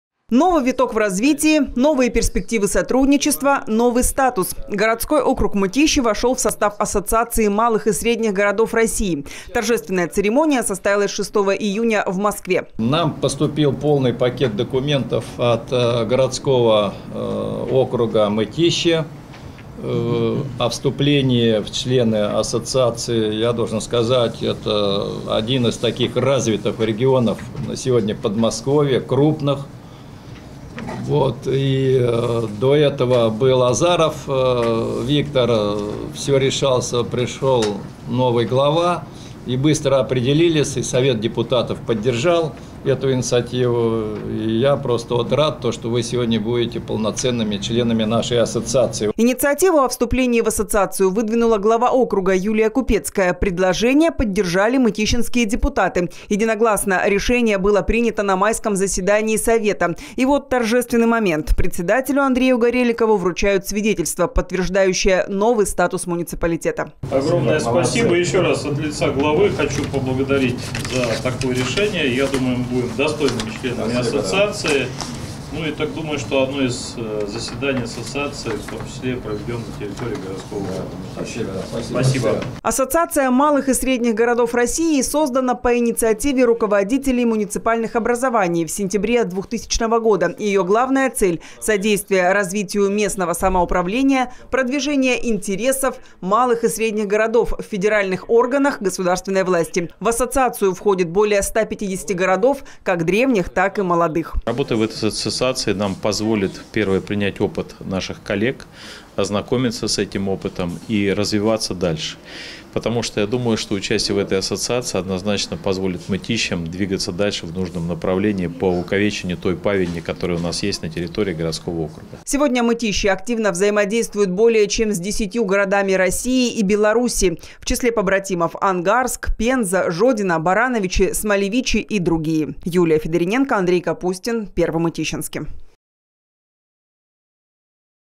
Мытищи вошли в состав Ассоциации малых и средних городов России
Городской округ Мытищи вошел в состав Ассоциации малых и средних городов России. Вручение Свидетельства о вступлении состоялось на заседании правления и попечительского Совета объединения. Ранее глава округа Юлия Купецкая выступила с инициативой пополнить ряды Ассоциации. Мытищинские депутаты идею поддержали, так как это содействует развитию местного самоуправления, продвижению интересов малых и средних небольших населенных пунктов в федеральных органах государственной власти. (12+)